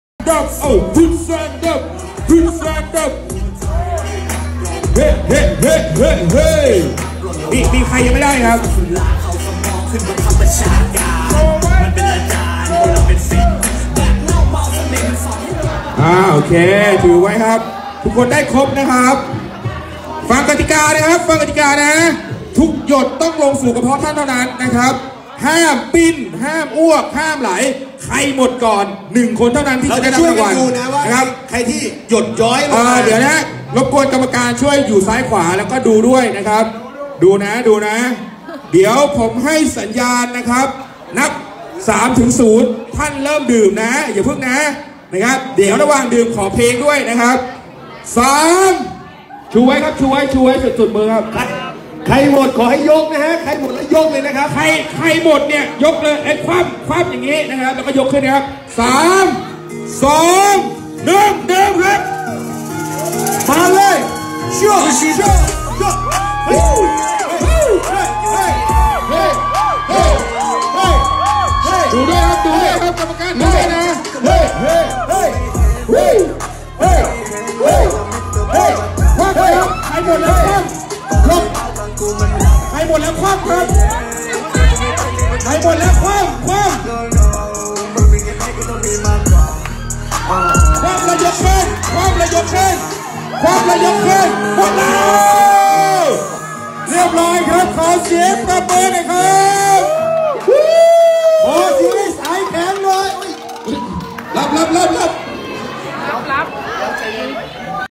Sujeto muere al tomar cerveza en 30 segundos
OLV